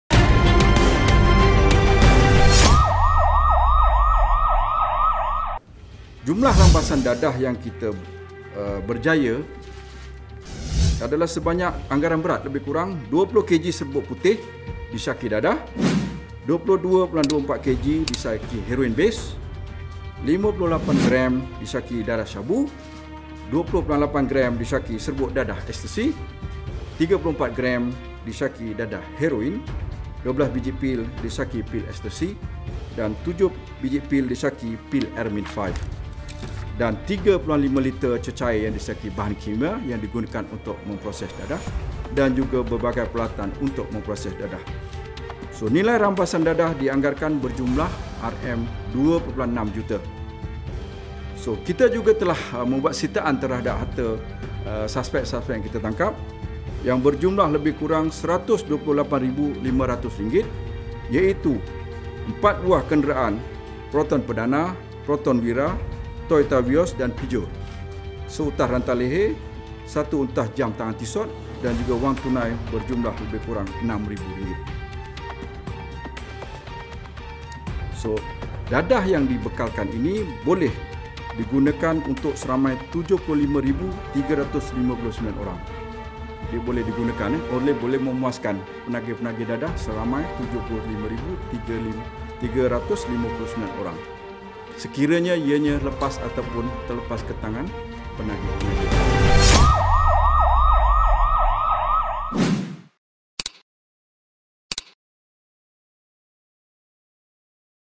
MKL Crimedesk | 7 Lelaki Pengedar Dadah Diberkas Polis
Sindiket pengedaran dadah tumpas lagi.

Kali ini tujuh lelaki, termasuk seorang warga iran diberkas dalam tiga serbuan berasingan di sekitar lembah klang .

Hasil serbuan polis merampas pelbagai jenis dadah bernilai 2.6 juta ringgit.

Ketujuh-tujuhnya yang berusia 33 hingga 56 tahun terbabit memproses dan mengedar dadah sejak dua bulan lalu.

Timbalan pengarah jabatan siasatan narkotik deputi komisioner zainudin ahmad berkata, semua suspek ditahan pada serbuan bermula 14 ogos hingga selasa lepas.

Dalam serbuan pertama, polis menahan empat lelaki di sebuah rumah di selayang baru, batu caves yang dijadikan makmal memproses dadah.

Dada serbuan kedua di kawasan sama, polis menahan dua lelaki, termasuk warga iran.

Serbuan ketiga pula membawa kepada penahanan seorang lagi suspek di sekitar lembah klang.

Polis  merampas 42.4 kilogram dadah seperti syabu, serbuk ekstasi, heroin, pil ekstasi, eramin 5 selain 3.5 liter cecair kimia.

Selain itu, polis turut merampas empat kereta iaitu proton perdana, proton wira, toyota vios dan peugeot bersama seutas rantai leher, jam tangan mewah serta wang tunai dengan nilai sitaan lebih 128 ribu ringgit.

Dadah itu dipercayai diperoleh dari china dan diproses di makmal terbabit untuk pasaran tempatan.